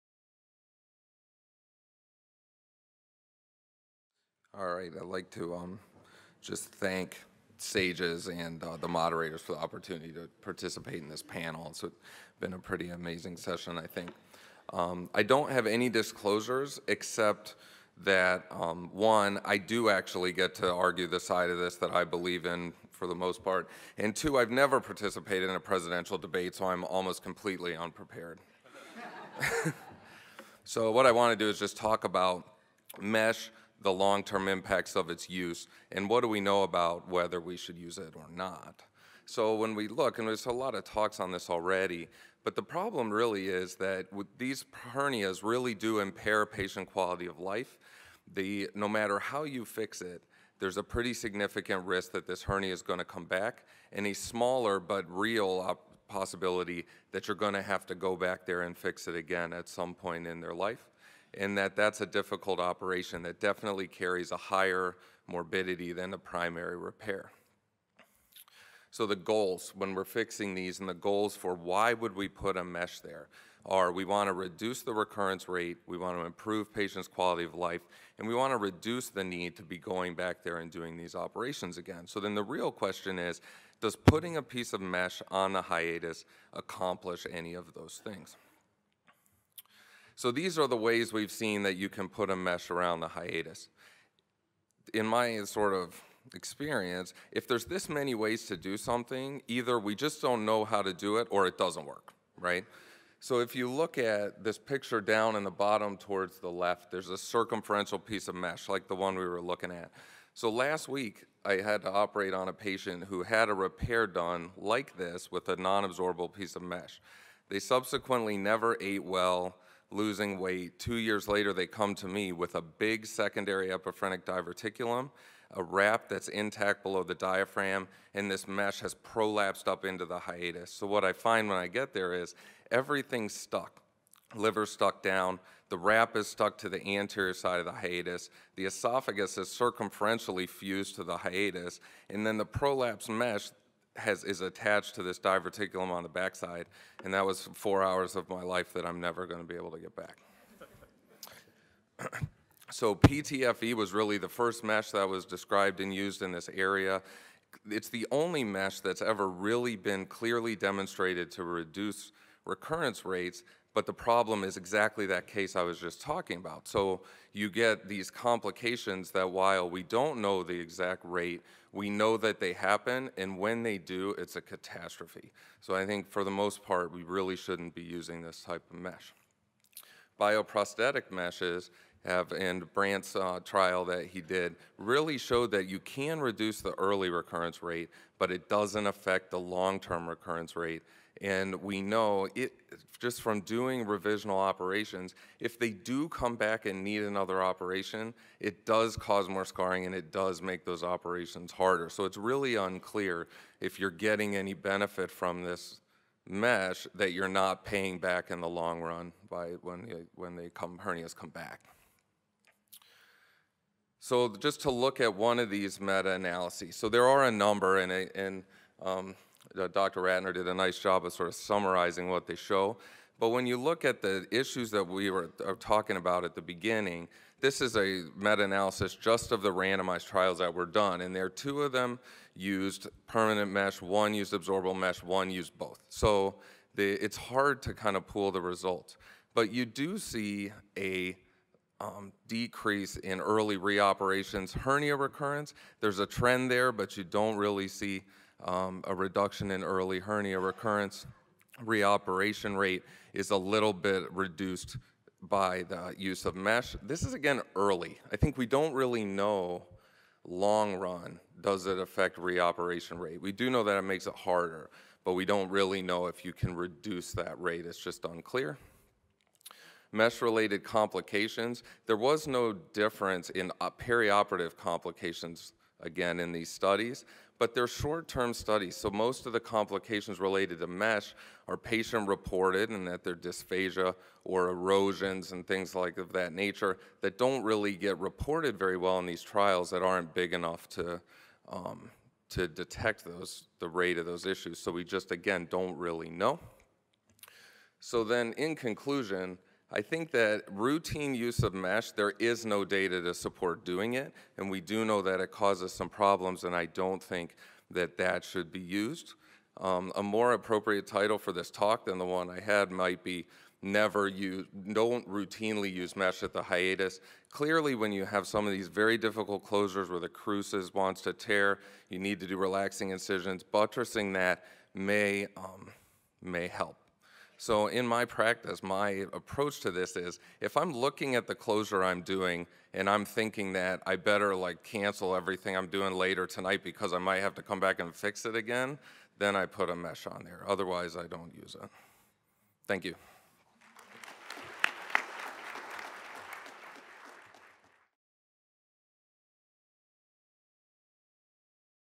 Never use mesh at the hiatus!
This talk was presented at the 2018 SAGES Meeting/16th World Congress of Endoscopic Surgery by Kyle A Perry during the Devil's in the Details: Paraesophageal Hernia Repair on April 11 2018